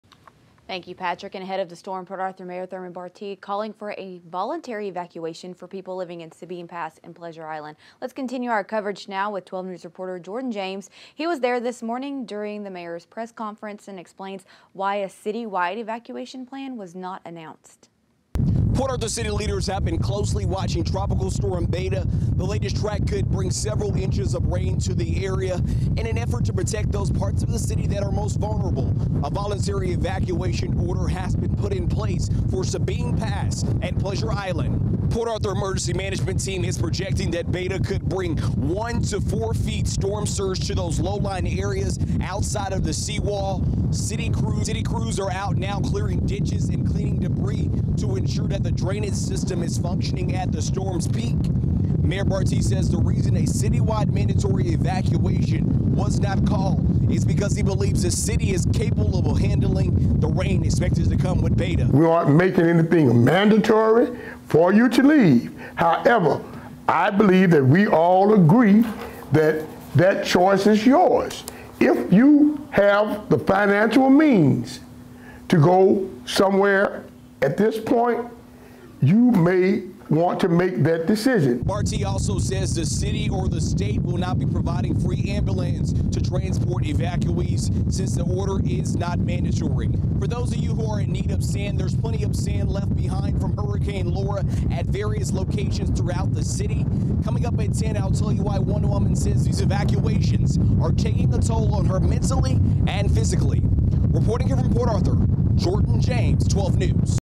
Port Arthur mayor calling for voluntary evacuation of Sabine Pass, Pleasure Island
City crews are working to make sure the drainage system is fully ready for rain that comes with Beta